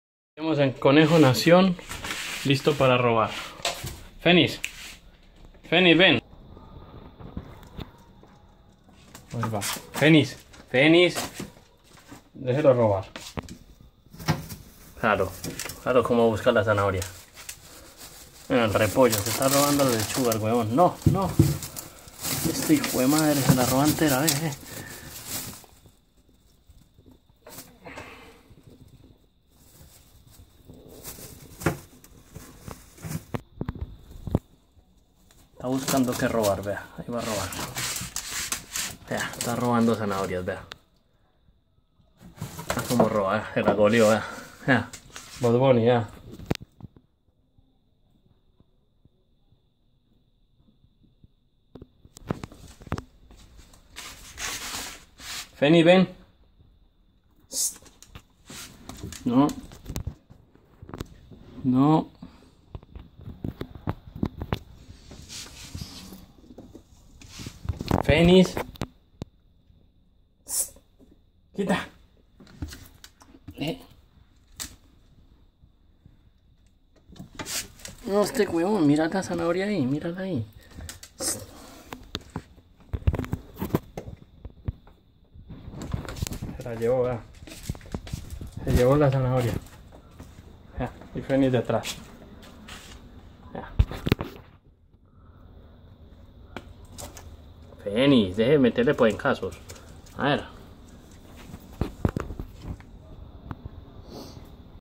Bugs y podenco fénix robando zanahoria
El conejo bugs robando zanahoria de la nevera jajaja.